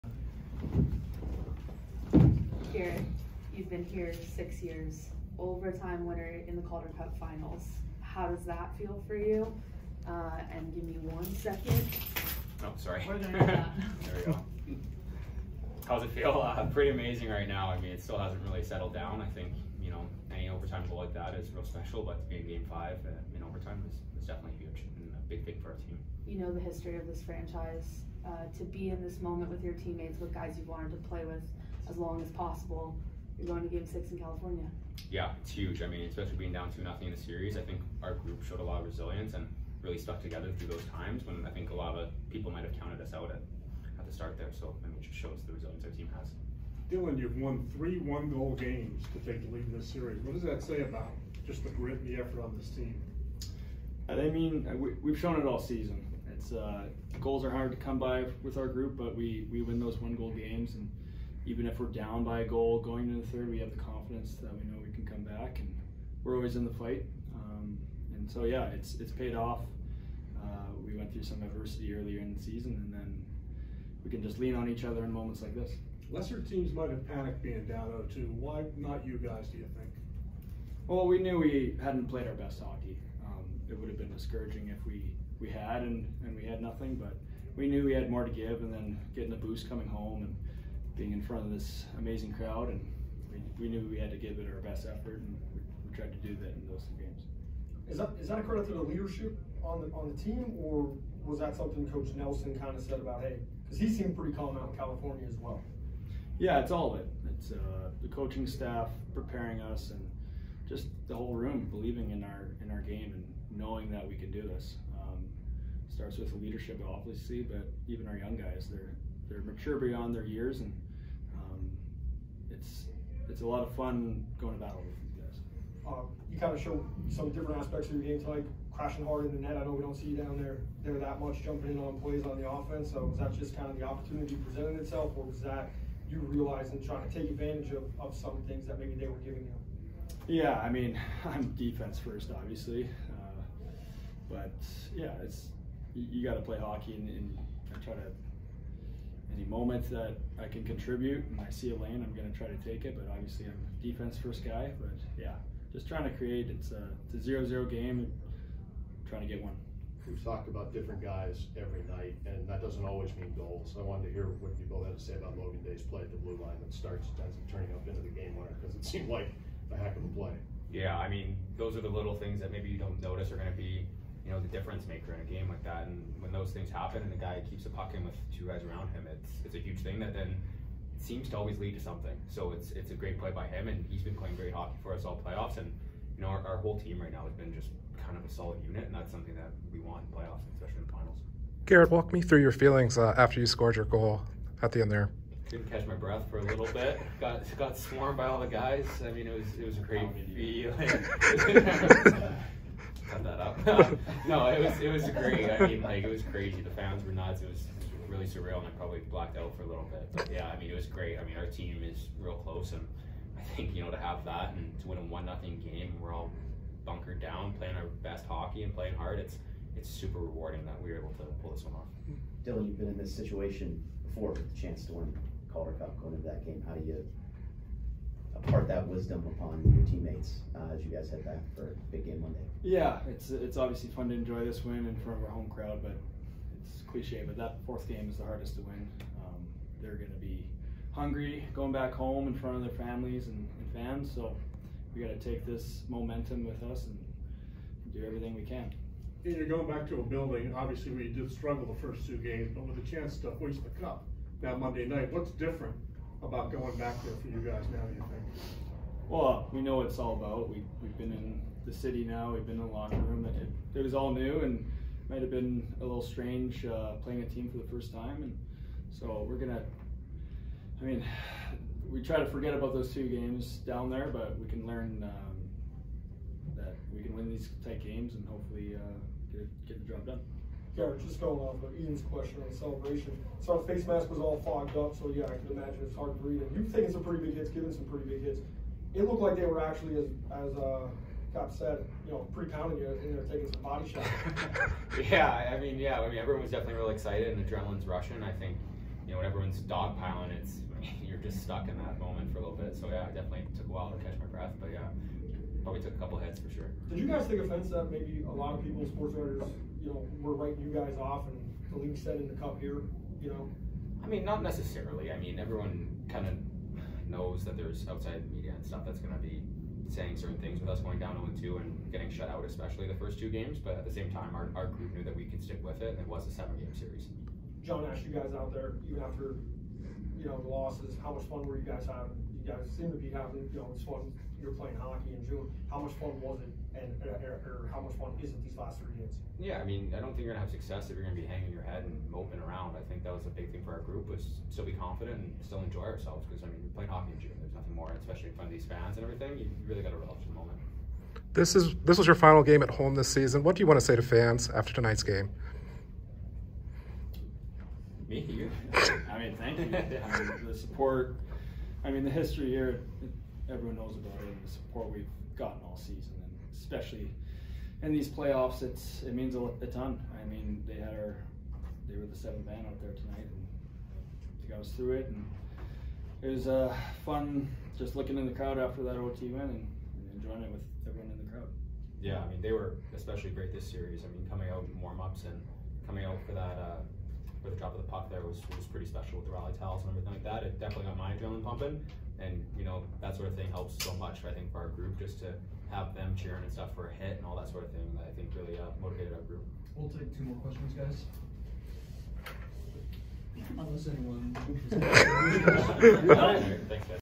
Garrett Pilon and Dylan McIlrath speak after Hershey's 1-0 OT victory in Game 5 of Calder Cup Finals
Garrett Pilon scored the OTGWG to give the Hershey Bears a 3-2 series lead in the 2023 Calder Cup Finals.